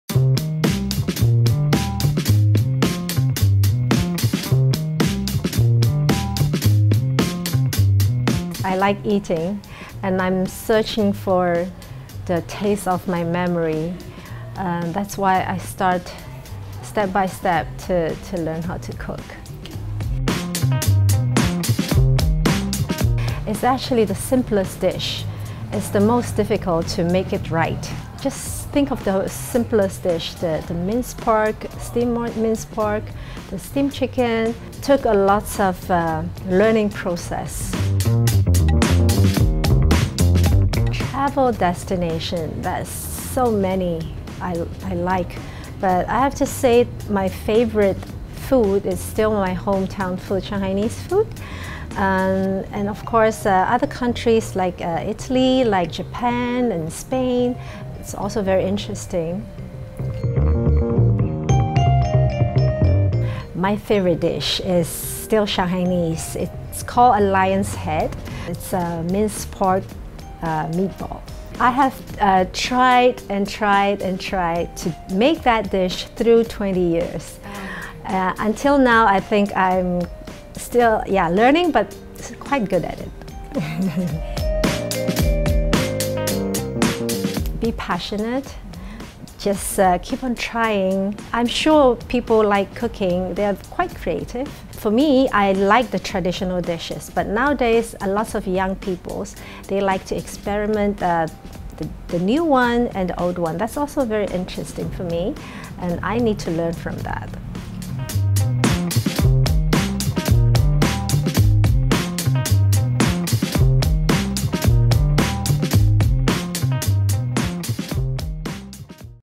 PrestigeTalks with Cover Star Socialite Chef Yvonne Yuen in April Issue
In prestigetravelandfoodissue, Cover Star Socialite Chef Yvonne Yuen found her passion of teaching people how to cook when she moved from Hong Kong to Indonesia. “When you can share your knowledge with other people, share what you like and they appreciate it – I feel really fulfilled,” she says. Read the full story,